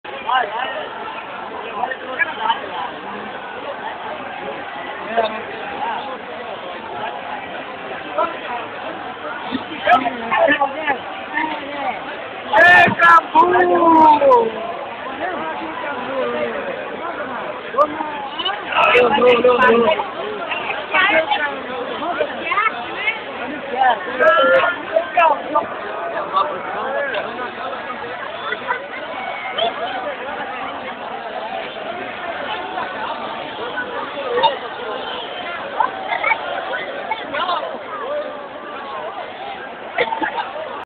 carretela festa do imigrante italiano santa tereza -ES
muito vinho ppolenta e linguiça tudo de graça e muita beleza que encantaram os turistas